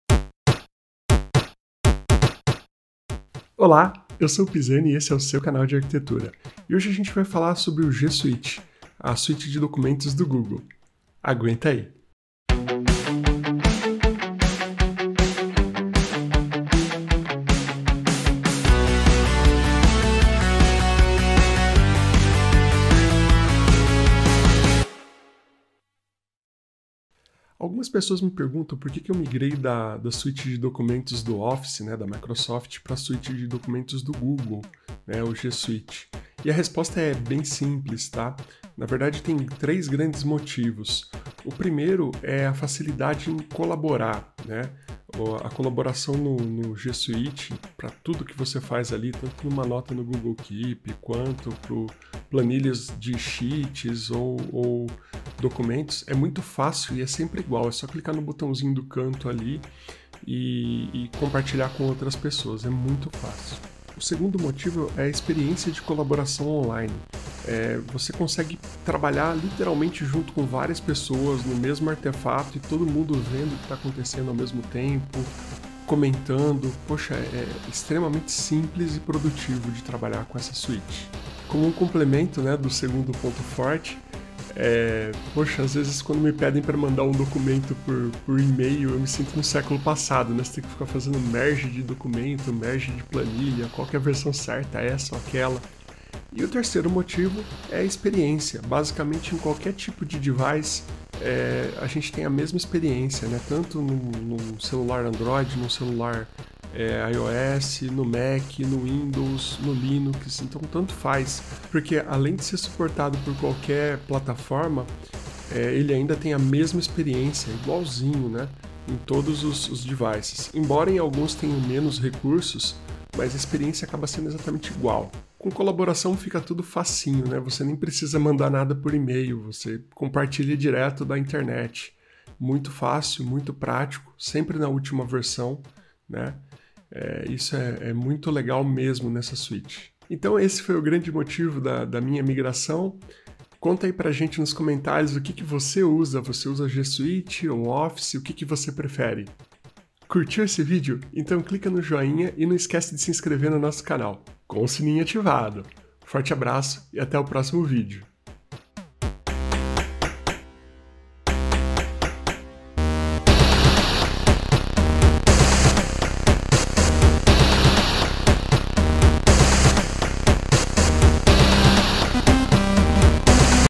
Motivos para migrar para G-Suite
Neste breve vídeo falamos dos principais diferenciais do G-Suite em relação a outras plataformas para escritório.